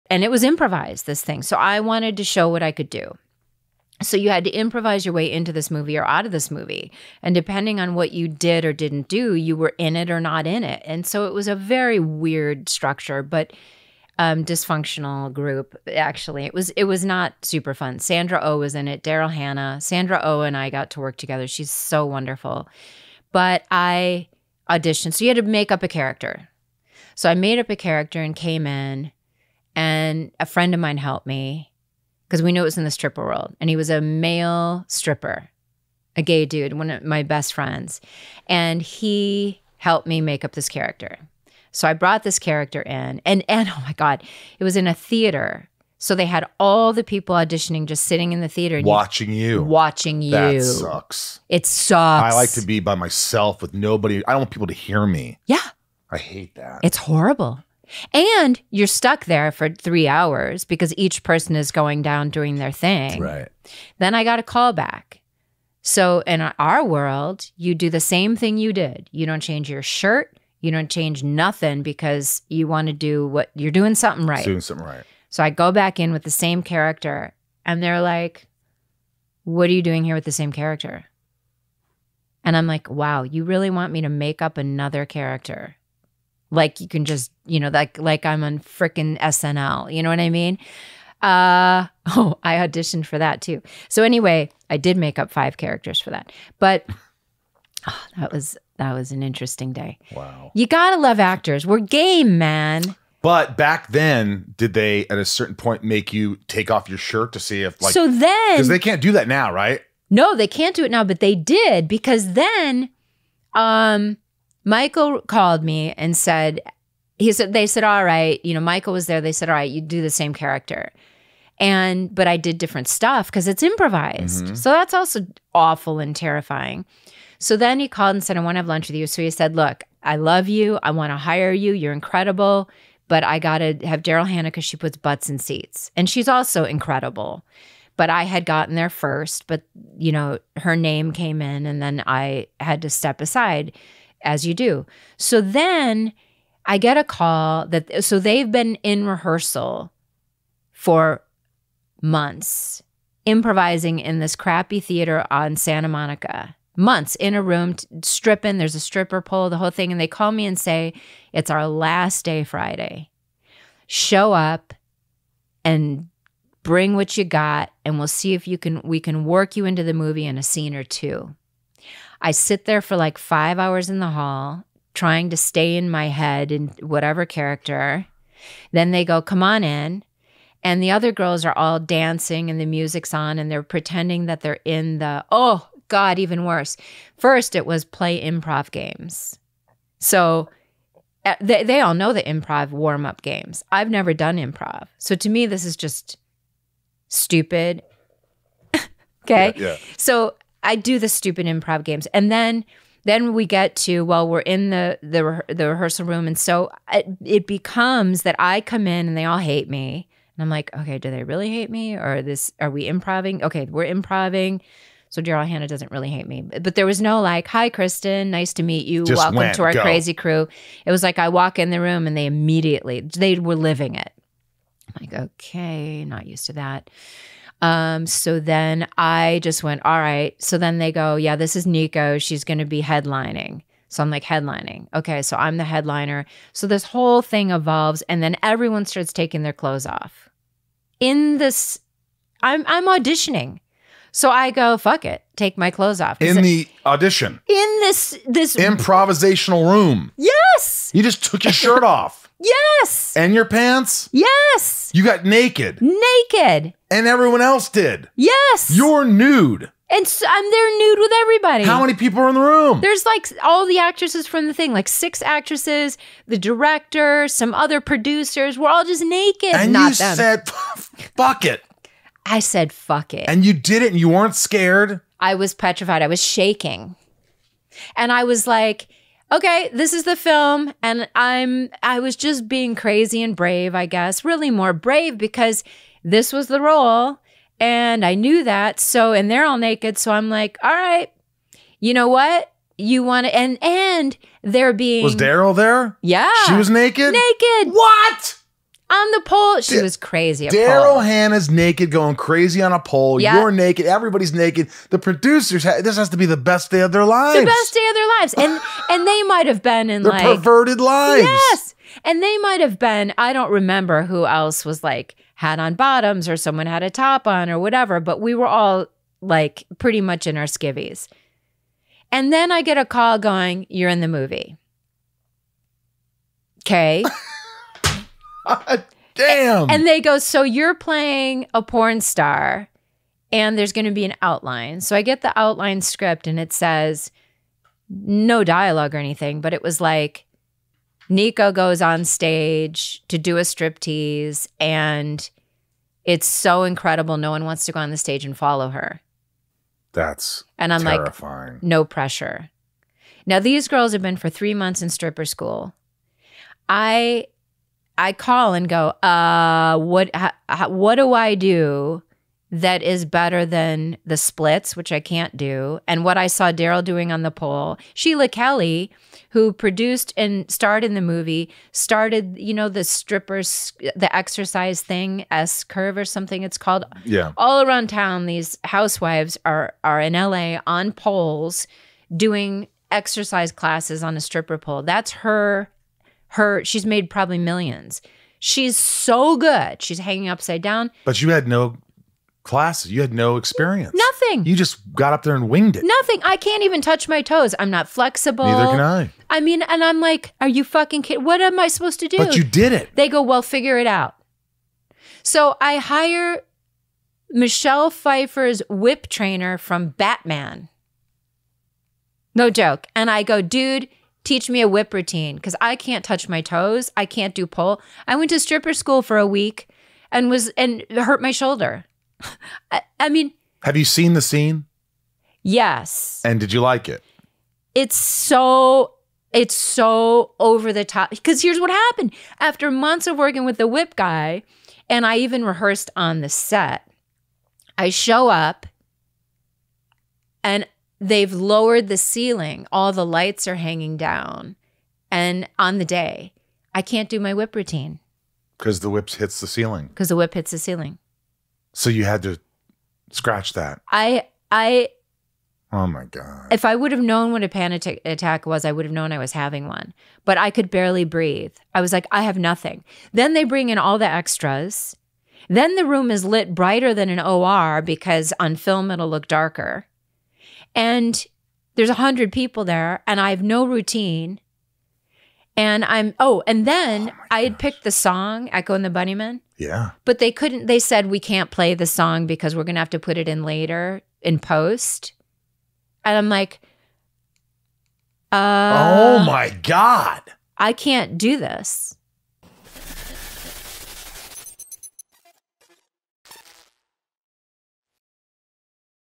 Whips… Panic Attack… and Nudity! KRISTIN BAUER Remembers a Wild Audition!!
Kristin Bauer (True Blood, The Boy Behind the Door) joins us this week to share her experience going from a small town upbringing in Wisconsin, to becoming a cosmetologist, and eventually falling in love with film and landing a dream role in True Blood. Kristin tells all this week, and opens up about some unfortunate experiences in this industry, including being fired off of HBO’s Hung and an extremely uncomfortable and risque audition for Dancing at the Blue Iguana. We also talk about ageism in Hollywood, her love for the crew and writing on a set, and how she’s able to pull from her emotions so easily during a performance.